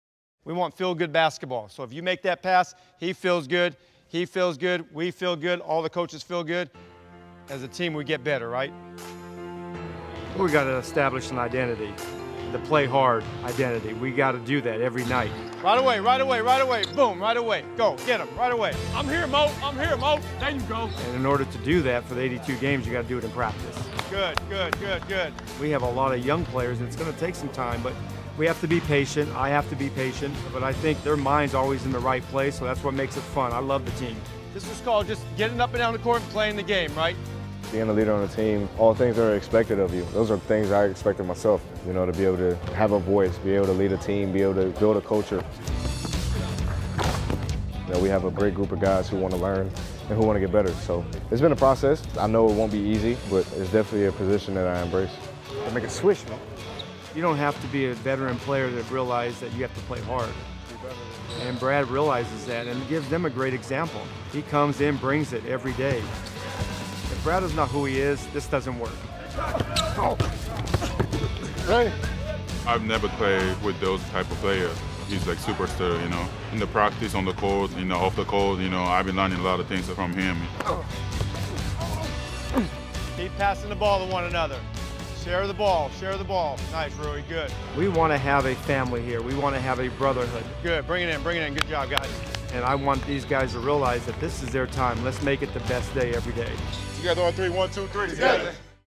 NBA All-Access: Washington Wizards Practice
NBA Entertainment takes you inside practice with the Wizards.